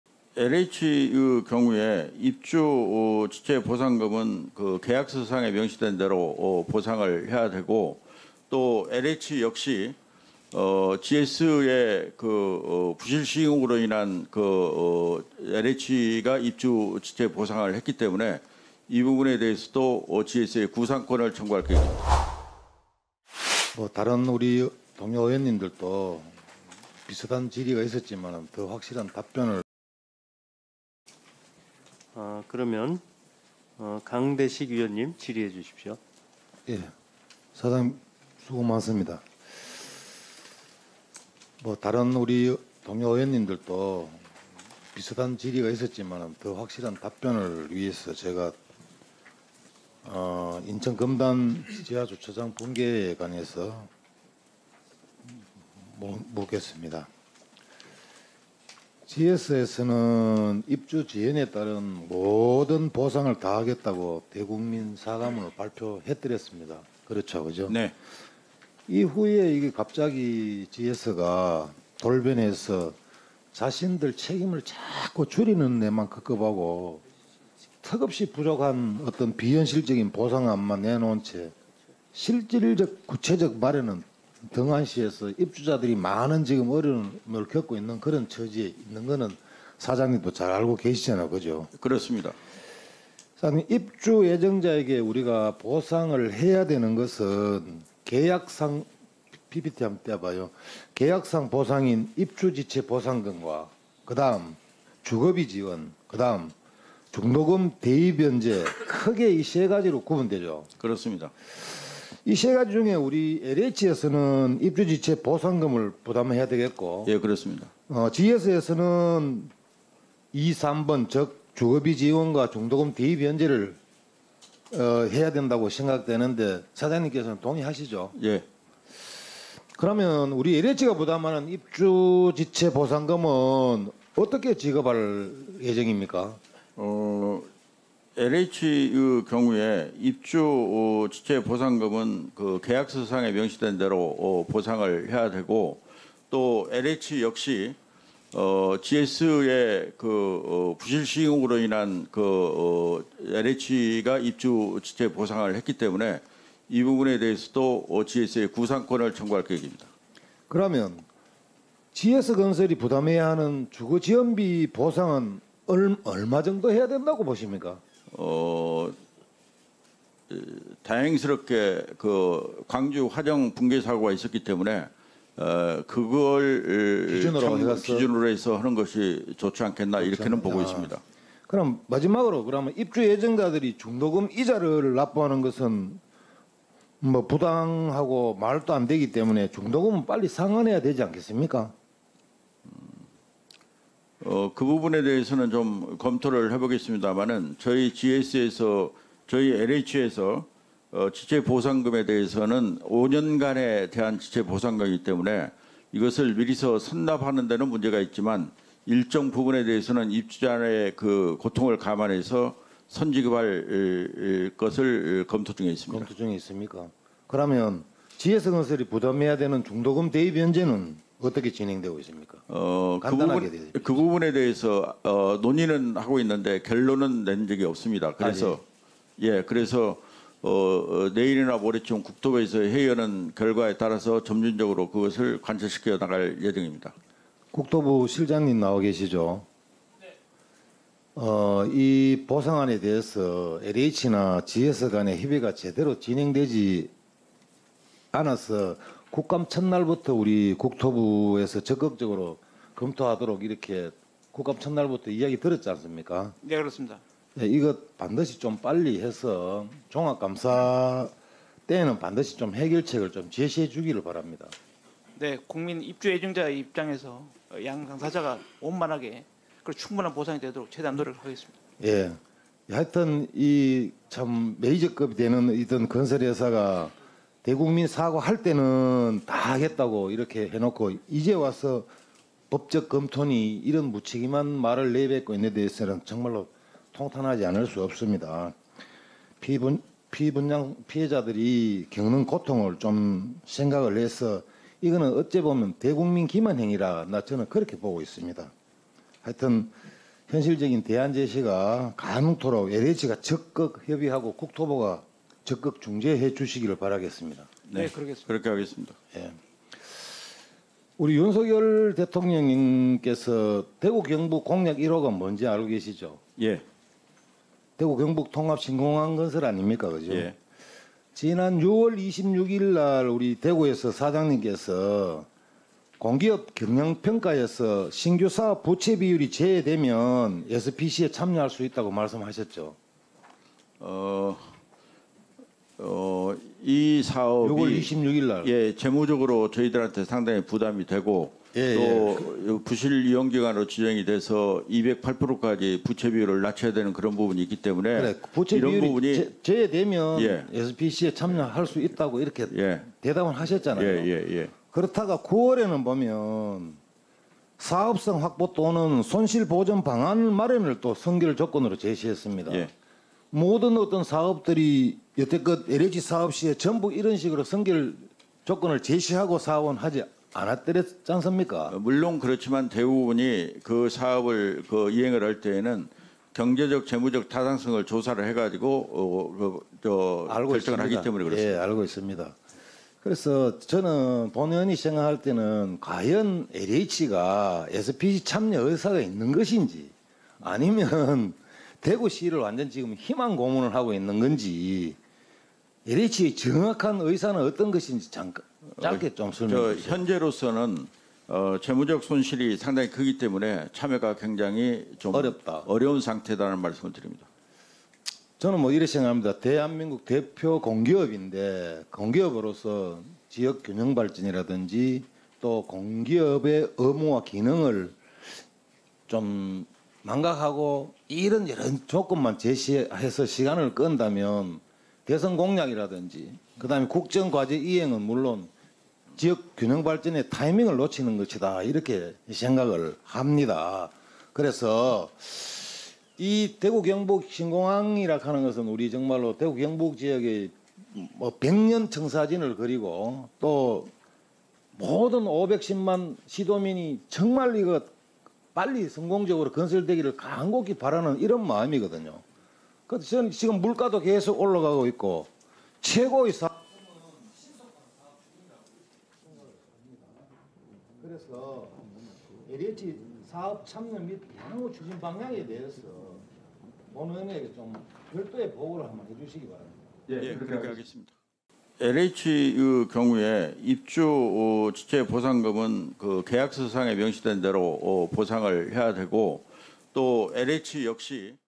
[2023년 국정감사] LH "검단 아파트 입주지체 보상금 지급…GS건설에 구상권 청구하겠다"
이한준 한국토지주택공사 사장이 16일 서울 여의도 국회에서 열린 국토교통위원회의 한국토지주택공사, 국토안전관리원 등에 대한 국정감사에서 지하주차장이 무너져 전면재시공이 결정된 인천 검단 아파트의 예비입주자들에게 입주지체보상금을 지급하고 GS건설에 구상권을 청구하겠다고 밝혔다.

이날 국민의힘 강대식 의원은 질의 시간에 "입주 예정자에게 보상해야 하는 것은 계약상의 보상인 입주지체 보상금과 주거비 지원, 중도금 대위변제 크게 세가지로 구분된다"며 "LH가 입주지체 보상금을 부담해야하고, GS건설은 주거비 지원과 중도금 대위변제를 해야한다"고 말했다.

이에 이 사장은 "입주지체 보상금은 계약서상에 명시된 대로 보상을 해야한다"면서 "GS건설의 부실시공으로 인해 LH가 입주지체 보상을 하기 때문에 이 부분에 대해서 GS건설에 구상권을 청구할 계획"이라고 답했다.

해당 클립은 이 사장을 상대로 국민의힘 강대식 의원의 질의 영상이다.

영상제공 국회방송